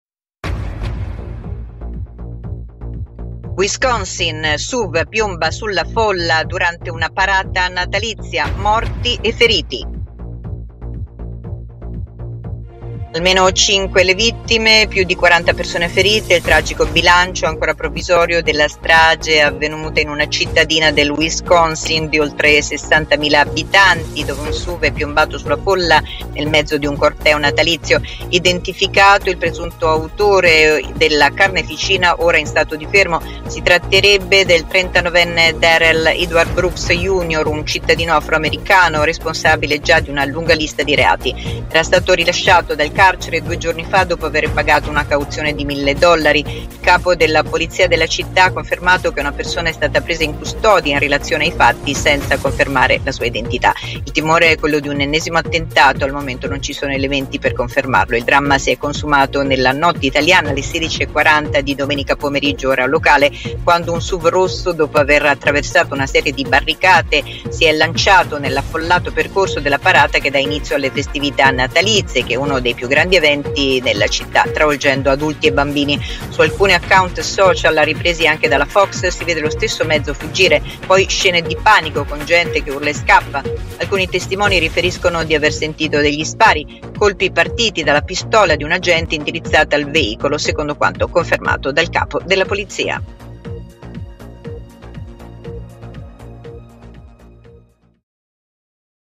Wisconsin, Suv piomba sulla folla durante una parata natalizia: morti e feriti
Almeno 5 vittime e più di 40 persone ferite. E' il tragico bilancio, ancora provvisorio, della strage avvenuta a Waukesha, una cittadina del Wisconsin di oltre 60 mila abitanti - a circa trenta chilometri da Milwaukee -  dove un Suv è piombato sulla folla nel mezzo di un corteo natalizio.